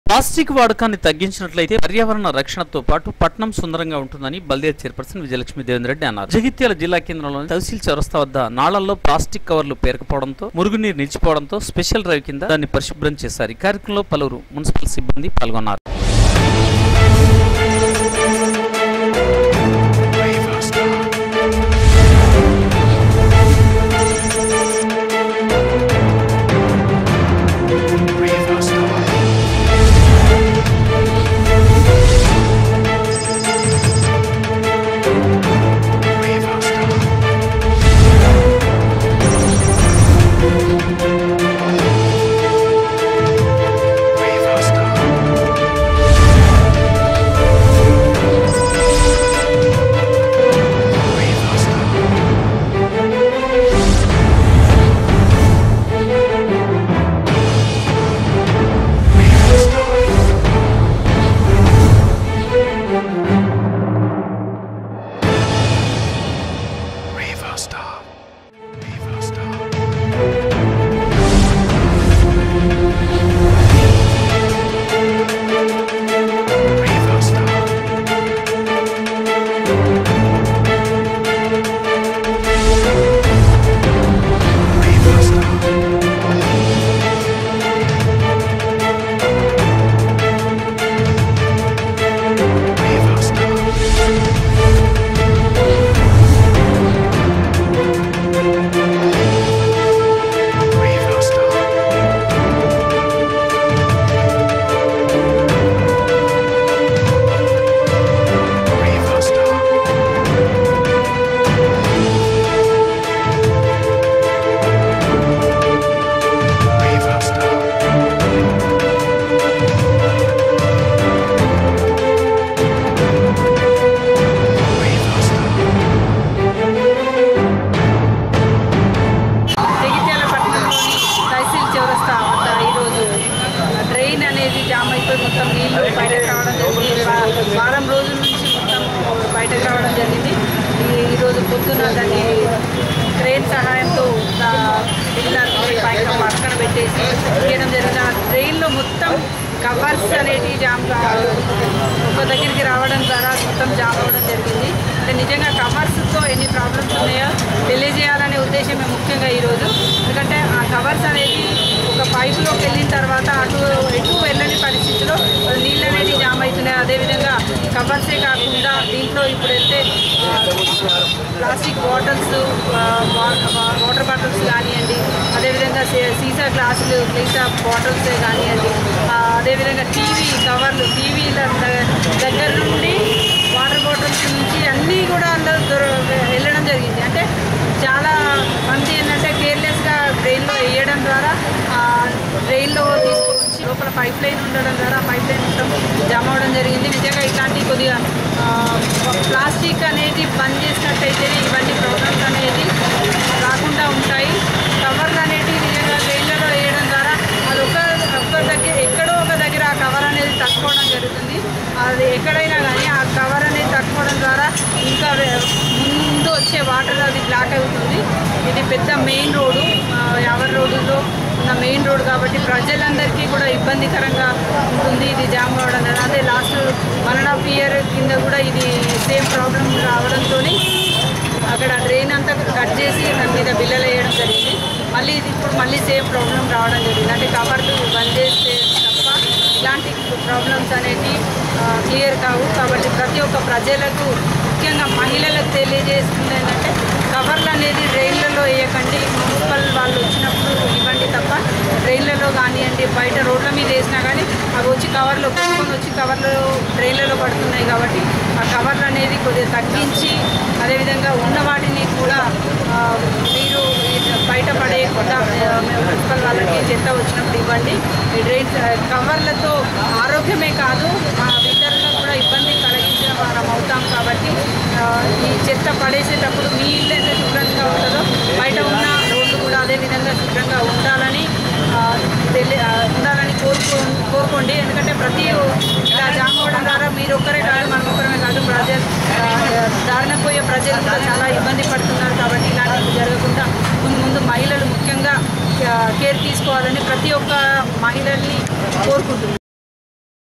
21.06.19| DSC | Jagtial |స్పెషల్ డ్రైవ్ తో మురికి కాలువలో నీ ప్లాస్టిక్ వ్యర్థాల తొలగింపు
| Durga Siti Cable | News |స్పెషల్ డ్రైవ్ తో మురికి కాలువలో నీ ప్లాస్టిక్ వ్యర్థాల తొలగింపు